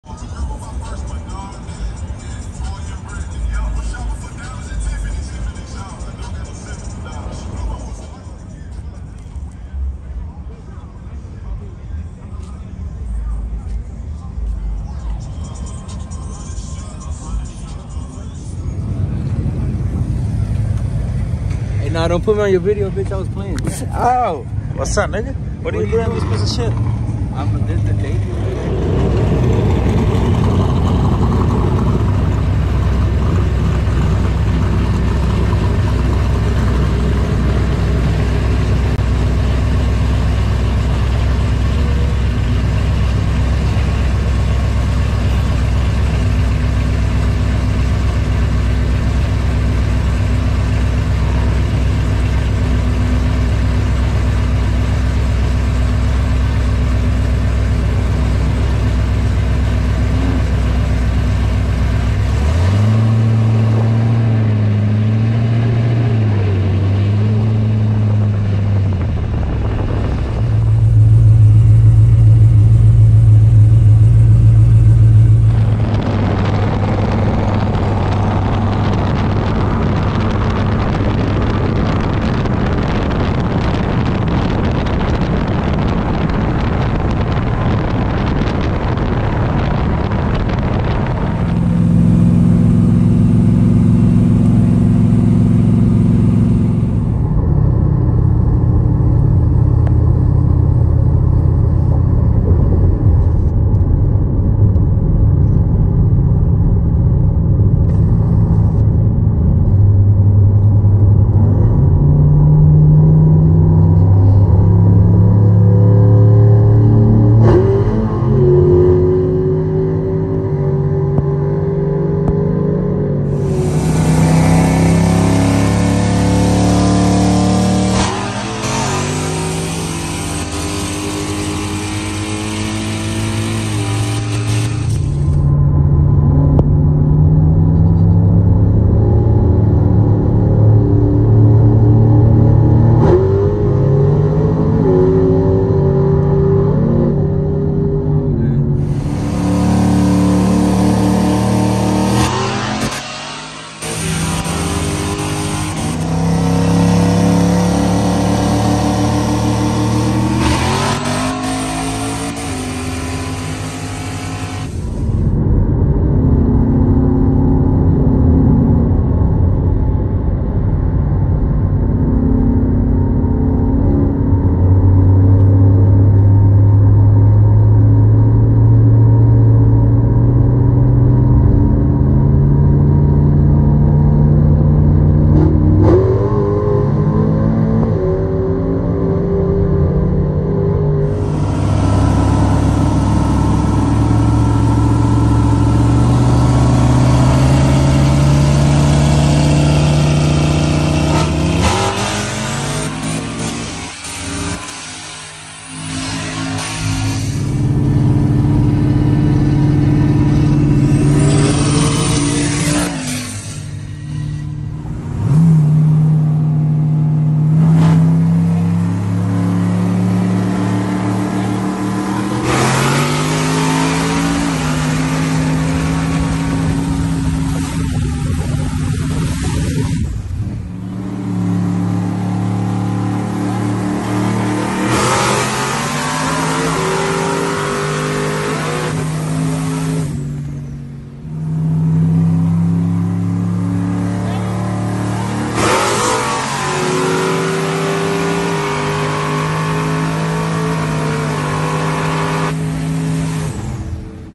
900 HP LSA SILVERADO VS 1K+ TURBO TRUCKS
Idk the what the other truck exactly has but runs pretty hard …for me win or lose is the same